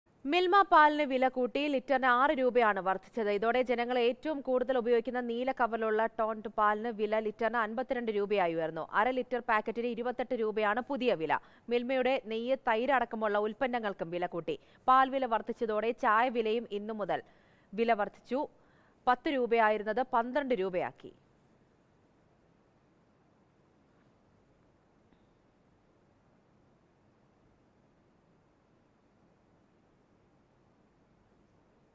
മിൽമ പാലിന് വില കൂടി; ചായയുടെ വിലയിലും വർധന | Mathrubhumi News
മിൽമ പാലിന് വില കൂടി. ലിറ്റിന് 6 രൂപയാണ് വർധിച്ചത്. ഇതോടെ ജനങ്ങൾ ഏറ്റവും കൂടുതൽ ഉപയോഗിക്കുന്ന നീല കവറിലുള്ള ടോൺഡ് പാലിന് വില ലിറ്ററിന് 52 രൂപയായി ഉയർന്നു..

Happy viewing!